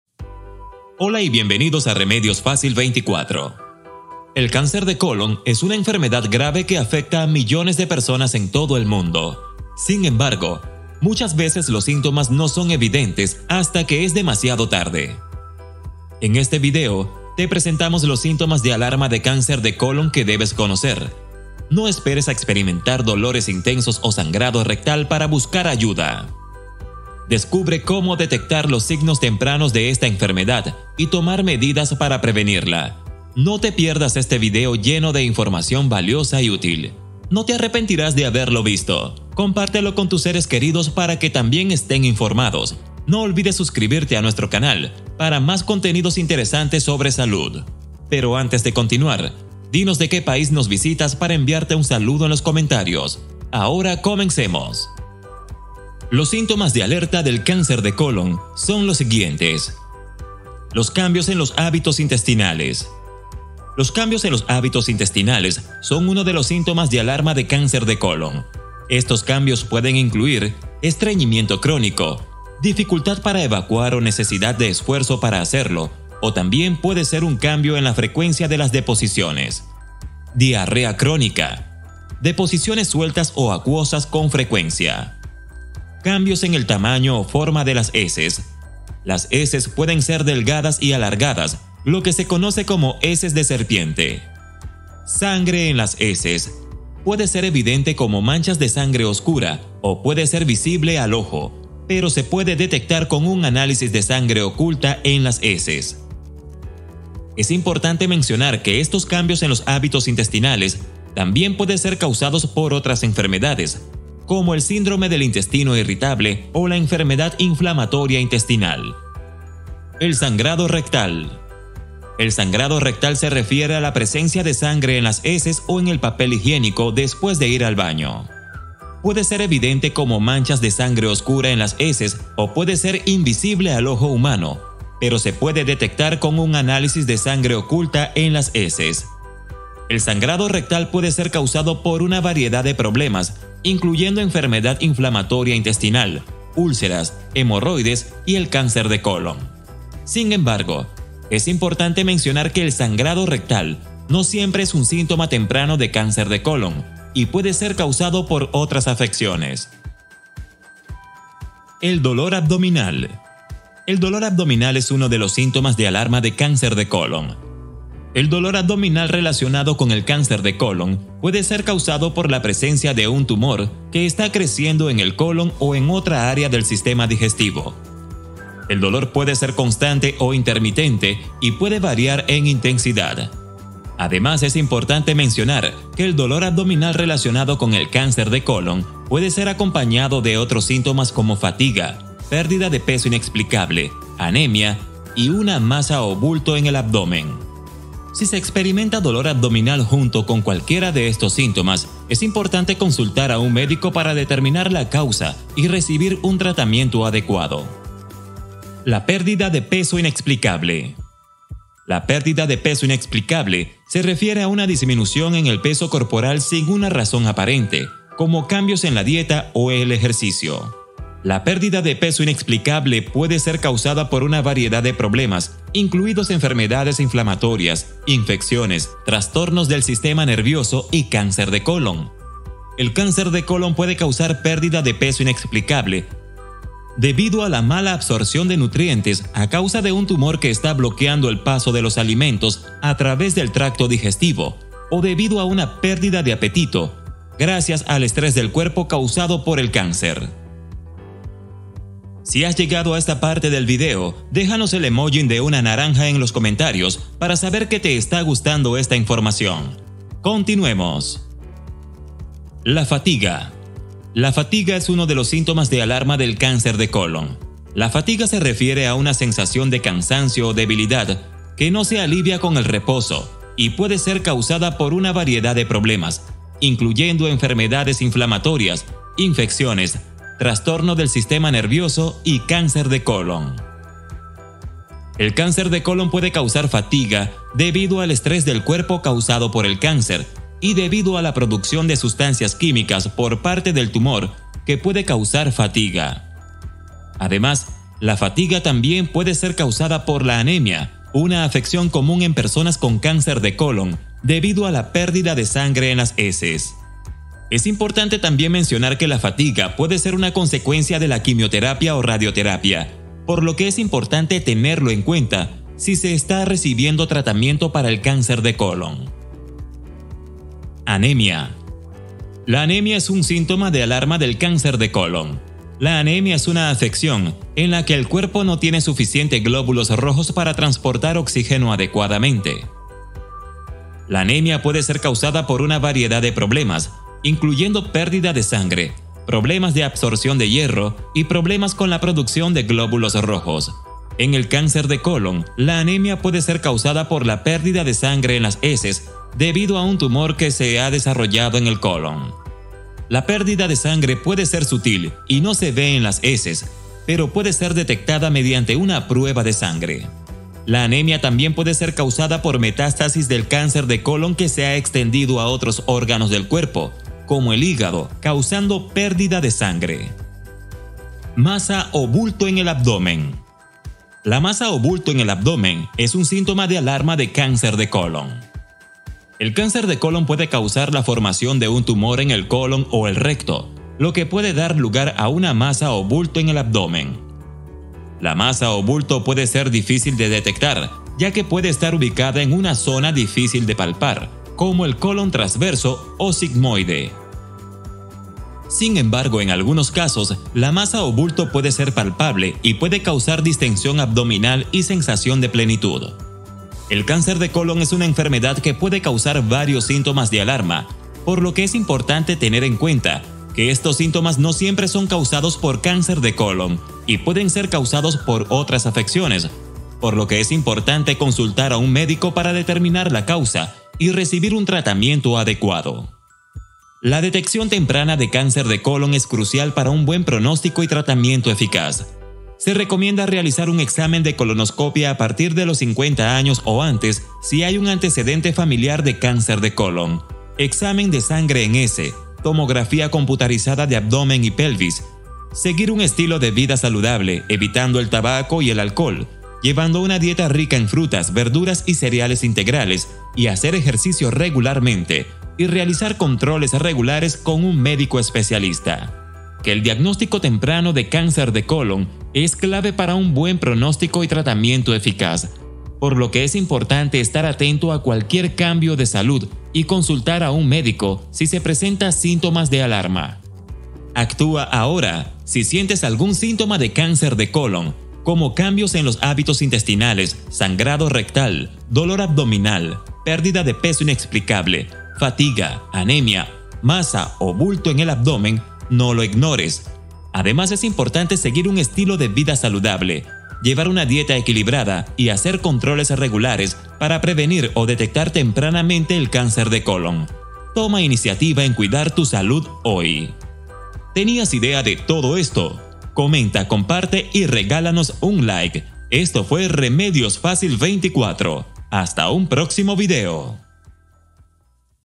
¡ALERTA! CONOCE LOS 5 SÍNTOMAS DE CANCER DE COLON ¡NO TE QUEDES SIN SABERLO!
👉¡Necesitas Ver Esto! TEMA
⏱️⏱️MARCAS DE VIDEO⏱️⏱️
00:00 - ¡ALERTA! CONOCE LOS 5 SÍNTOMAS DE CANCER DE COLON ¡NO TE QUEDES SIN SABERLO!
1:00 - LOS CAMBIOS EN LOS HÁBITOS INTESTINALES
2:00 - EL SANGRADO RECTAL
2:41 - EL DOLOR ABDOMINAL
3:28 - LA PÉRDIDA DE PESO INEXPLICABLE
4:22 - LA FATIGA
5:20 - LA ANEMIA
6:11 - LA MASA O BULTO EN EL ABDOMEN
6:46 - RECOMENDACIONES FINALES
8:24 - DESPEDIDA

El cáncer de colon es una enfermedad grave que afecta a millones de personas en todo el mundo.
Sin embargo, muchas veces los síntomas no son evidentes hasta que es demasiado tarde.
En este video, te presentamos los síntomas de alarma del cáncer de colon que debes conocer. No esperes a experimentar dolores intensos o sangrado rectal para buscar ayuda.
Descubre cómo detectar los signos tempranos de esta enfermedad y tomar medidas para prevenirla. ¡No te pierdas este vídeo lleno de información valiosa y útil!

🔴✅ ATENCIÓN: Este es un Video de Información y Consejos, por lo que  No Sustituye a un Especialista ni a Profesionales en Materia.